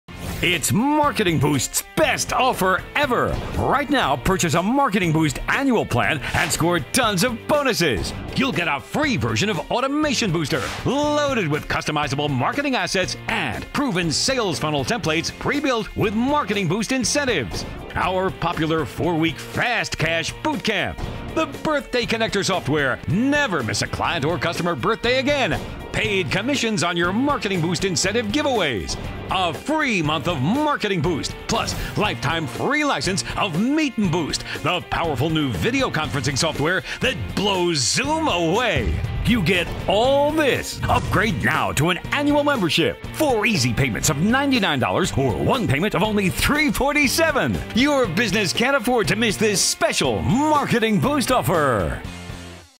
Marketing Boost Best Deal EVER for Unlimited Travel Incentives to Grow Your Business.
Our BEST Marketing Boost Deal EVER!  You’ll get a FREE version of Automation Booster- LOADED with customizable marketing assets-  AND proven sales funnel templates, pre-built with Marketing Boost incentives!

- Our popular 4-week FAST CASH Boot Camp!!

- The Birthday Connector software- NEVER miss a client or customer birthday again!

- A FREE month of Marketing Boost!!  -plus lifetime FREE license of “Meet-N-Boost”! –the powerful new video conferencing software the blows Zoom away!!

You get ALL this-  with your annual membership.

Upgrade NOW to Marketing Boost annual membership!
4 easy payments of $99, or one payment of $347!!

Your business can’t afford to miss this Best Deal EVER!!